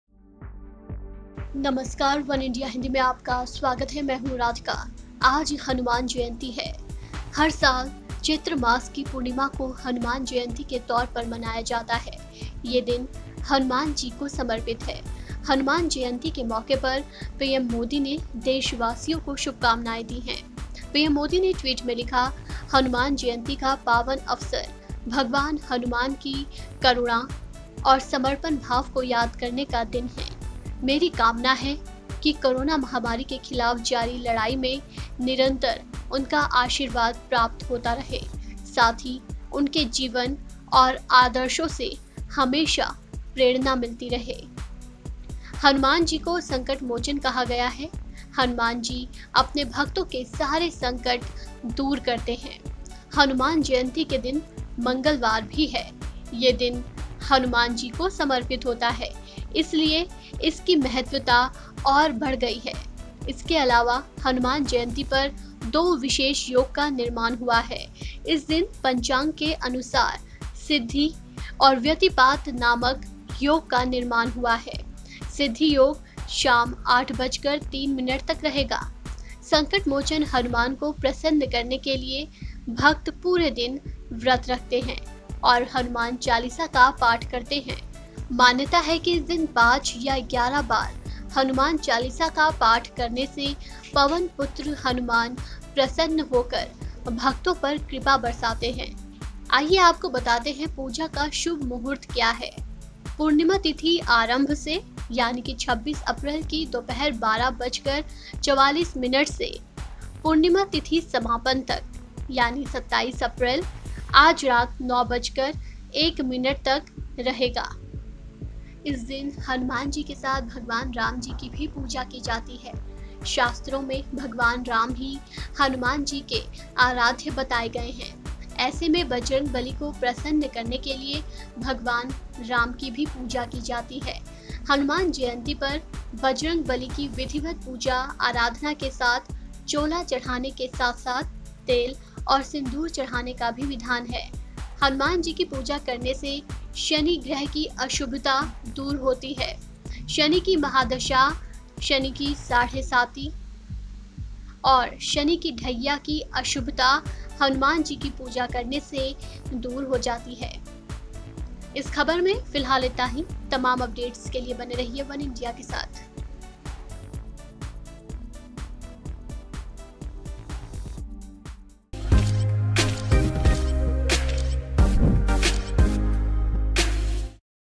Hanuman Jayanti 2021: Hanuman Jayanti आज, जानें शुभ मुहूर्त और पूजा का महत्व । वनइंडिया हिंदी
Hanuman Jayanti’ is celebrated on full moon day during Chaitra month to commemorate the birth of Lord Hanuman. The festival of Hanuman Jayanti is celebrated across the country with a lot of fervour and enthusiasm. Hanuman Jayanti 2021 marks the birth anniversary of Lord Hanuman, who is one of the most worshipped deities by Hindu devotees.

आज हनुमान जयंती है। हर वर्ष चैत्र मास की पूर्णिमा को हनुमान जयंती के तौर पर मनाया जाता है। ये दिन हनुमान जी को समर्पित है. हनुमान जयंती के मौके पर पीएम मोदी, ने देशवासियों को शुभकामनाएं दी हैं. पीएम मोदी ने ट्वीट में लिखा, "हनुमान जयंती का पावन अवसर भगवान हनुमान की करुणा और समर्पण भाव को याद करने का दिन है. मेरी कामना है कि कोरोना महामारी के खिलाफ जारी लड़ाई में निरंतर उनका आशीर्वाद प्राप्त होता रहे. साथ ही उनके जीवन और आदर्शों से हमेशा प्रेरणा मिलती रहे."